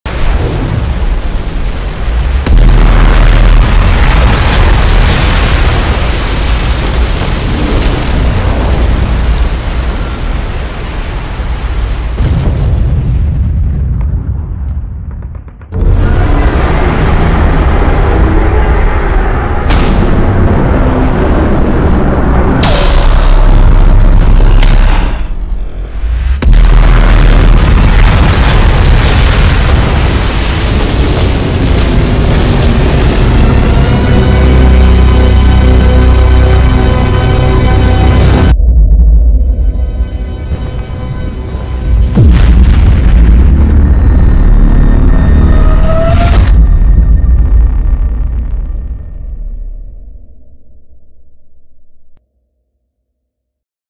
the skibidi wars 59 (legacy)
This episode is no longer canon to The Skibidi Wars universe. The beginning of the series will be revamped, and unfold in the planned prequels that will be created once the Necromancer Arc comes to a conclusion. The canon episode that is currently episode 76 will be turned into episode 26 once the prequels starts. The prequels will contain The Skibidi Wars Episode 1-25, and aims to explain everything in regards to the origins of the Tri-Titan, Toilet Emperor, Necromancer, The Alliance itself etc.